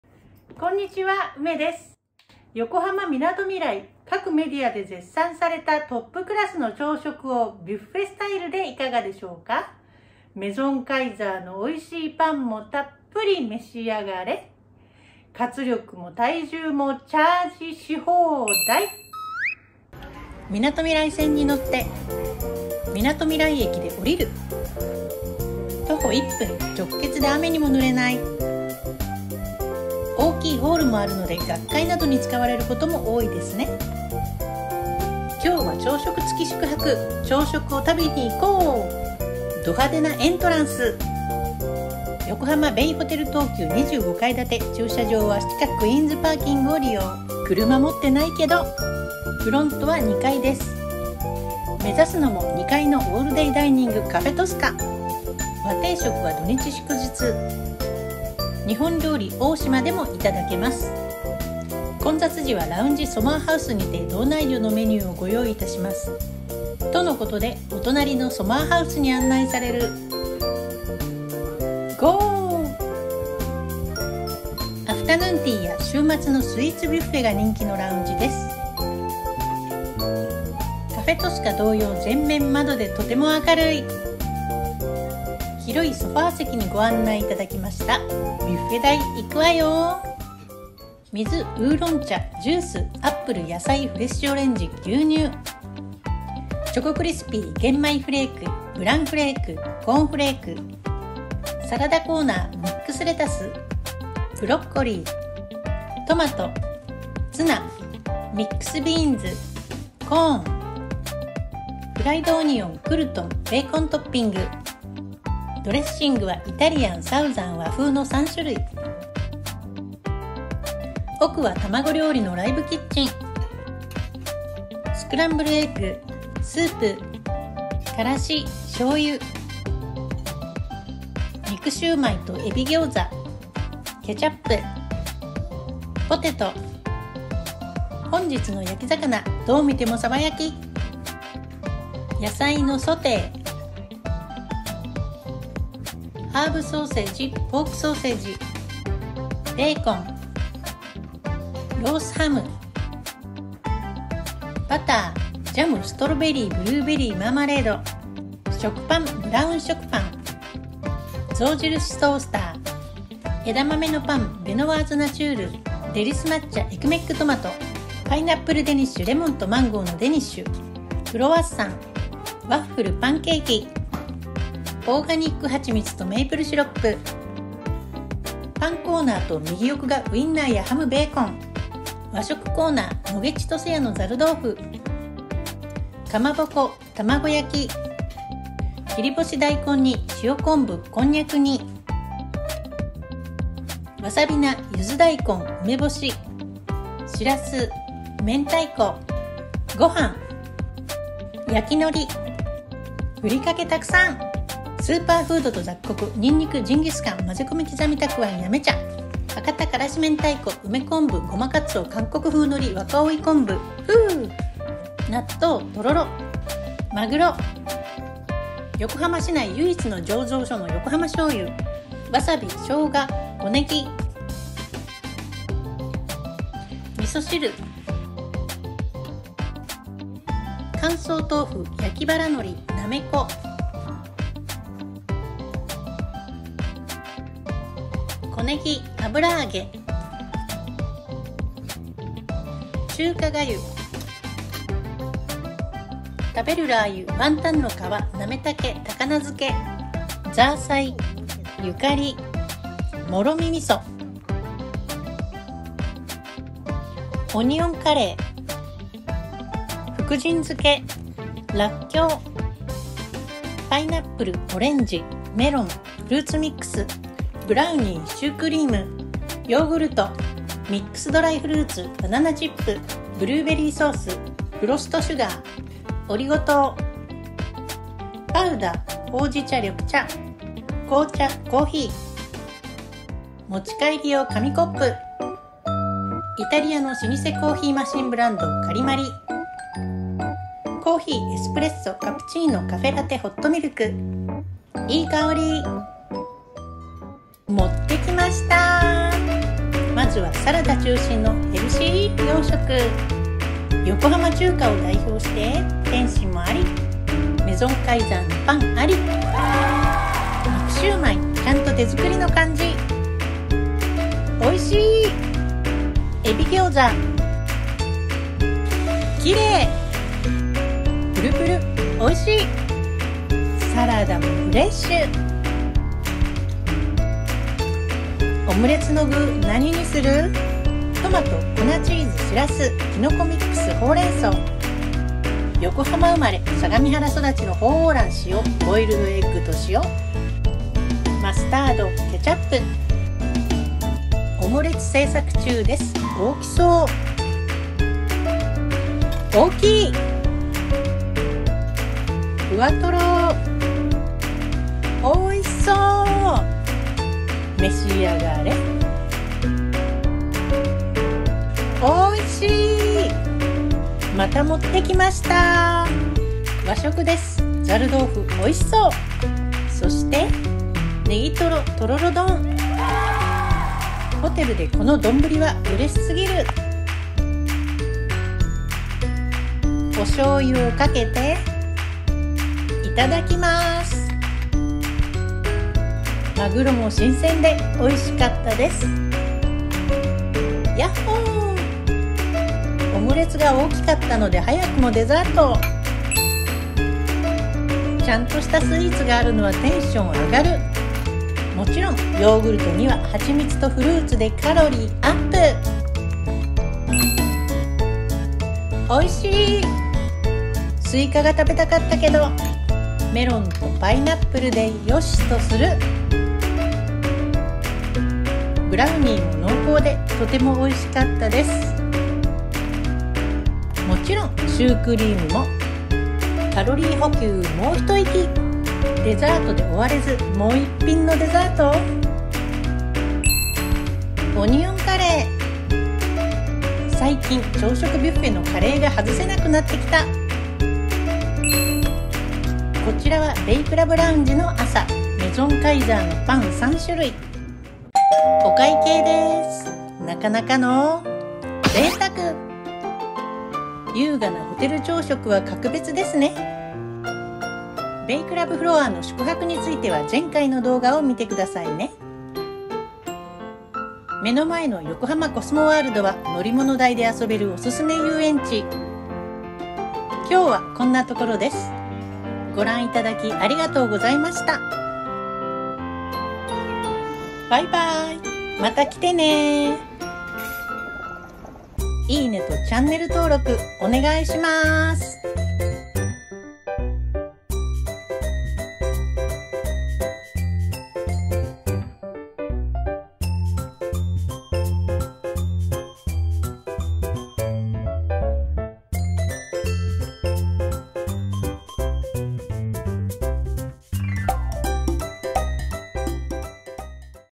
【横浜ベイホテル東急】豪華な朝食ビュッフェ 横浜 日本
ホテル朝食で活力とカロリーをチャージ！
和洋中で大満足に食べ過ぎましょう！？